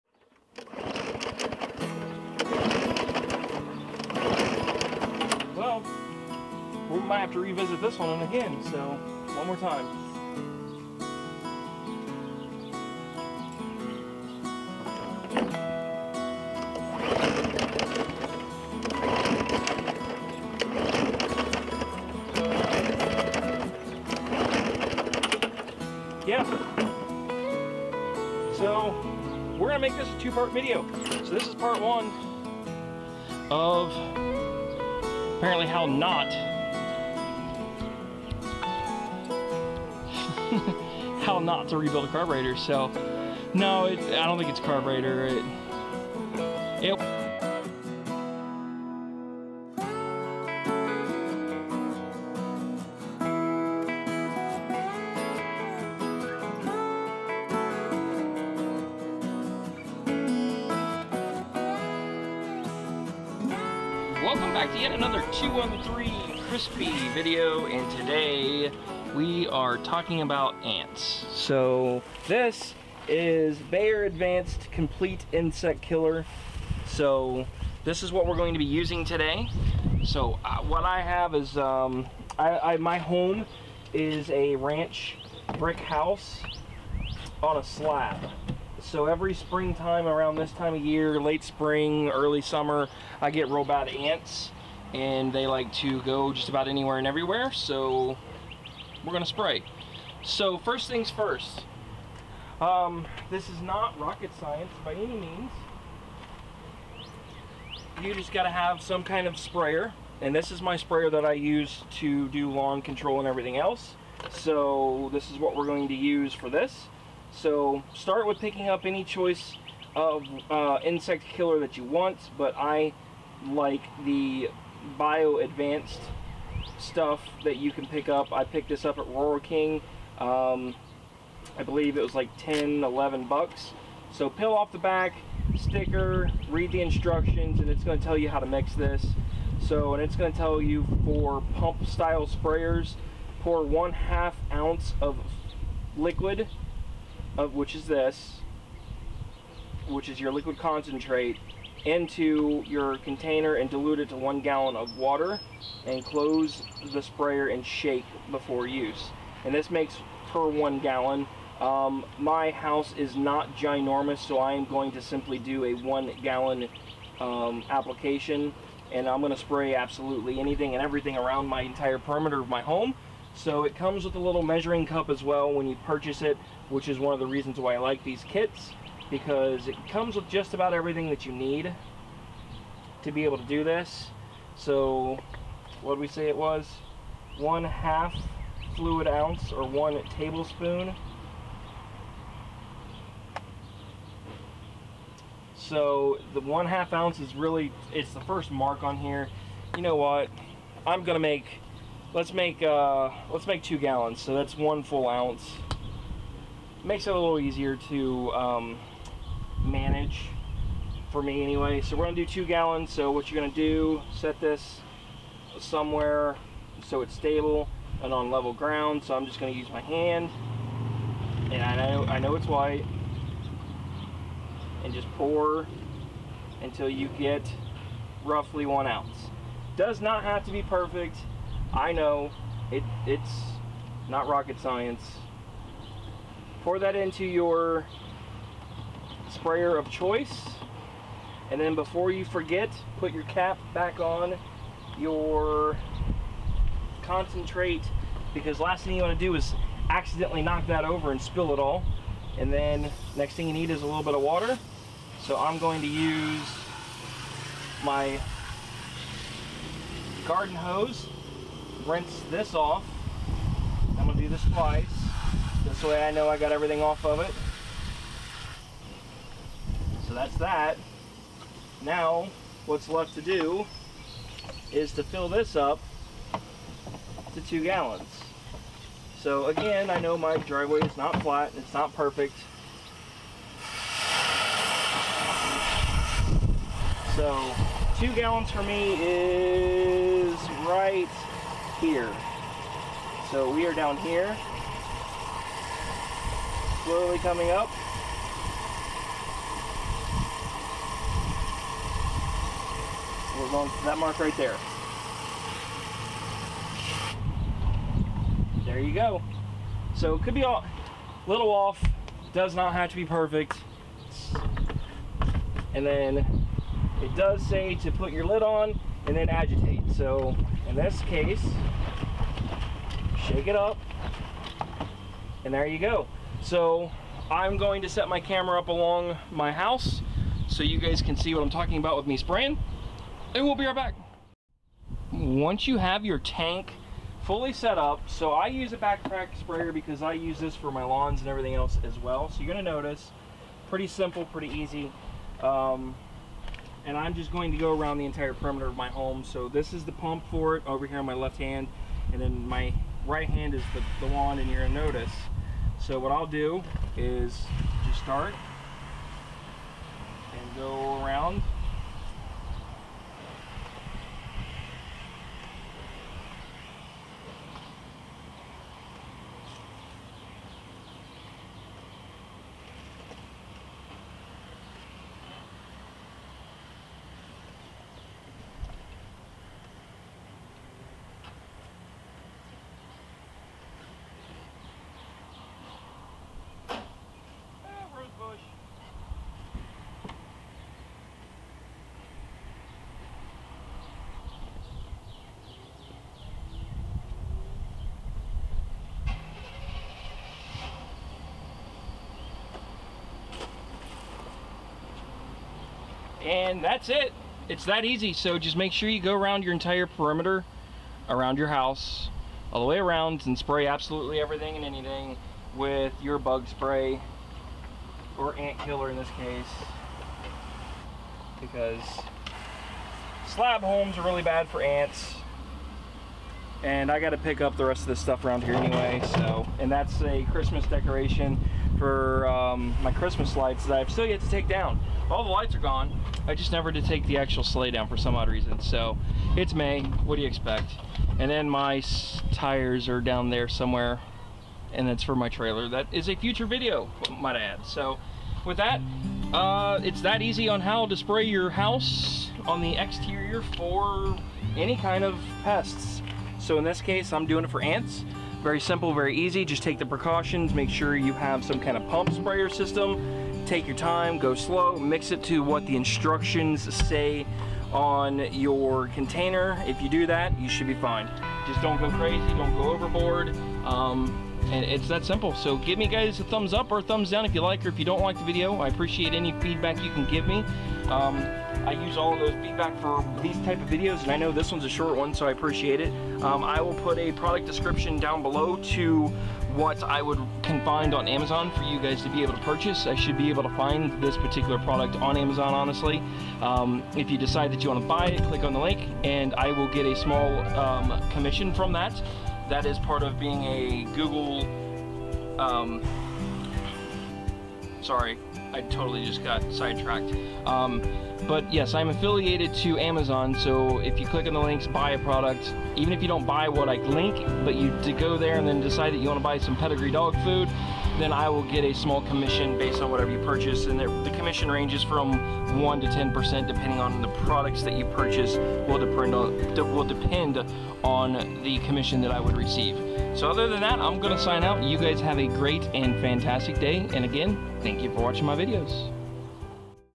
ants invade my house! lets kill them using bioadvanced complete insect killer
Simple how to video on how to spray the foundation of your home to prevent ants from coming into your house.
Sadly every late spring, my slab home will get a small trail of ants , so i have to spray , and this is how you spray your house for ants.

You simply spray around the foundation of the home to prevent a barrier that the ants do not like to go threw, which prevents the ants from coming into the home.

products used are linked blow.

Ant Mixture =